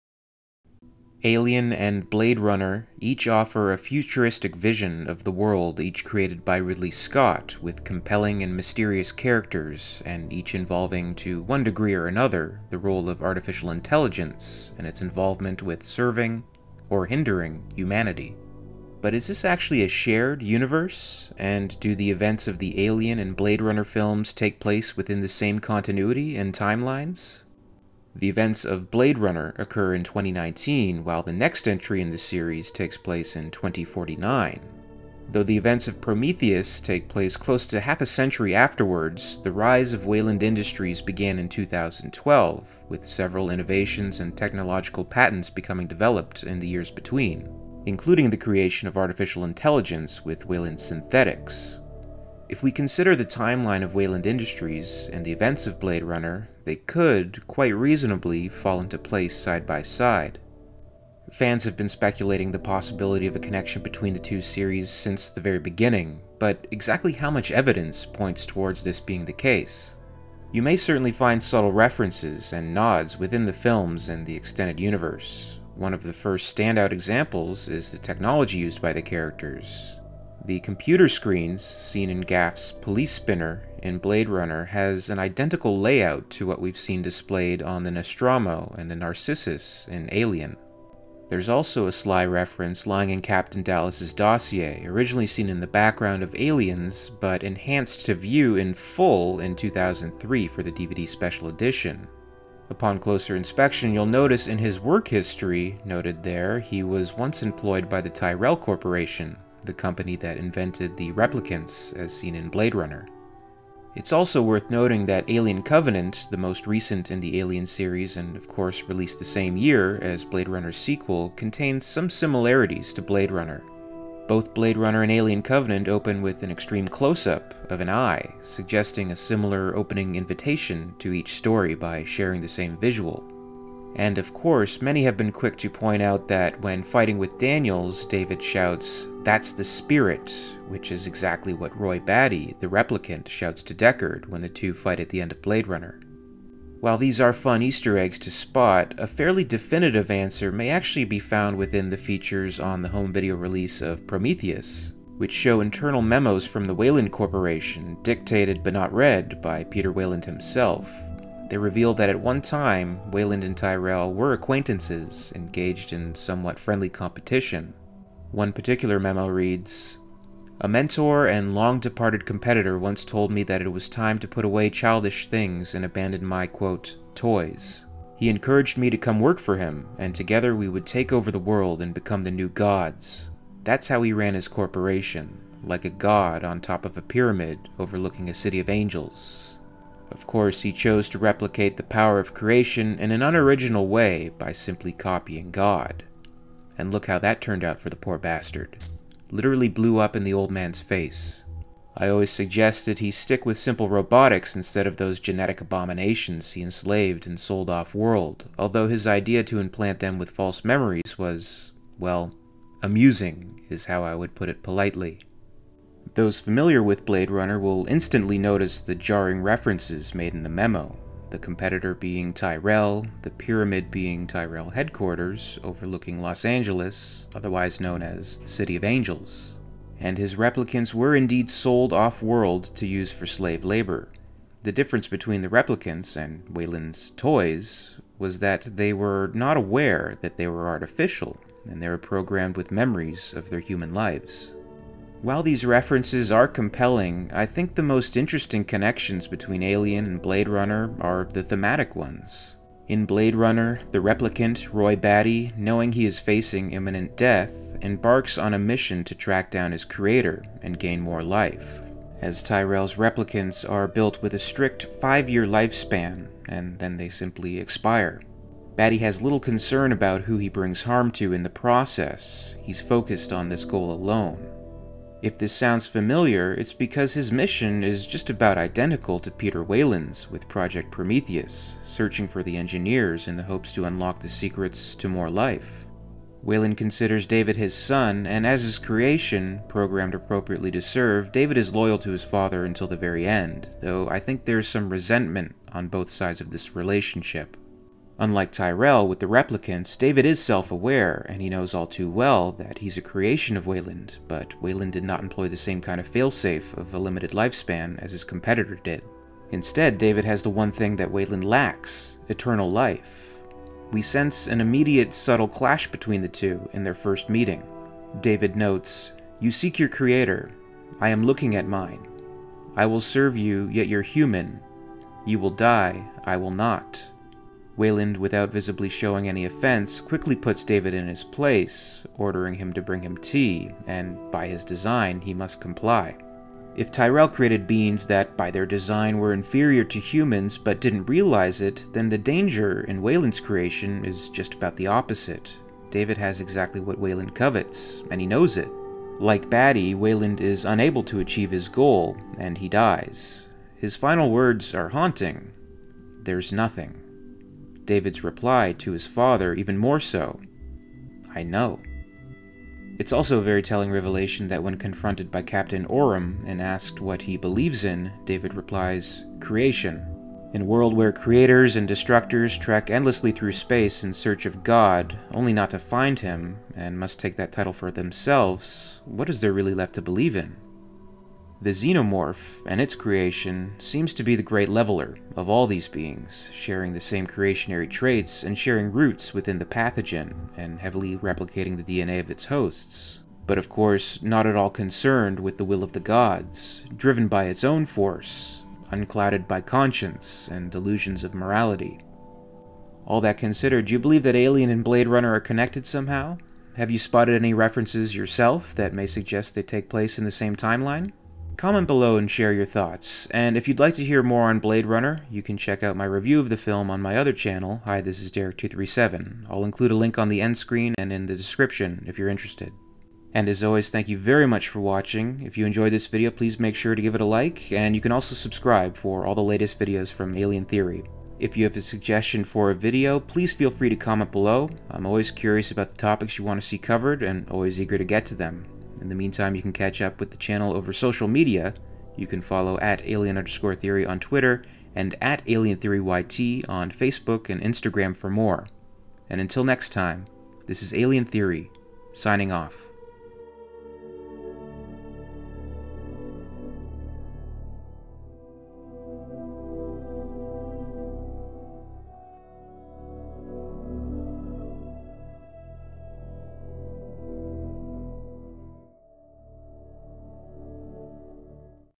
Do Alien and Blade Runner Exist in the Same Universe? - Theory
For years, fans have been looking for connections between the Alien series and Blade Runner. This video explores references, Easter Eggs, and themes that may support this theory.